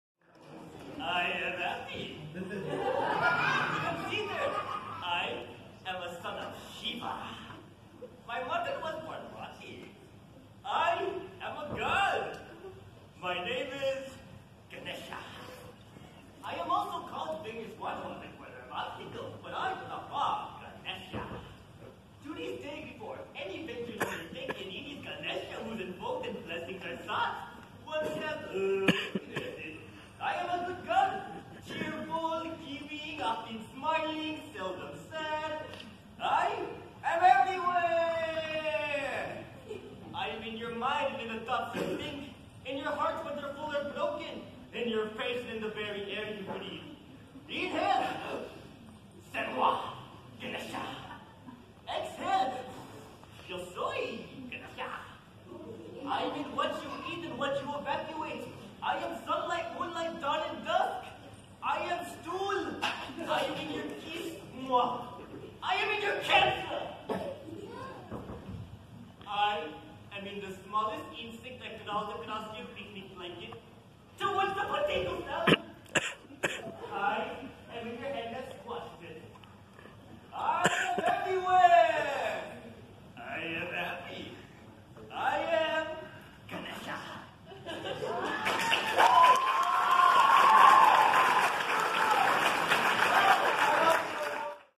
Ganesha (Theatre Monologue)
This is my performance of a monologue from a published play entitled, "A Pefect Ganesh." This play was written by Terrence McNally. This monologue is about the great and powerful Hindu God, Ganehsa, explaining his eternal existance. This monologue was performed at Columbia High School, located in Lake City, Florida, during one of its open mic nights!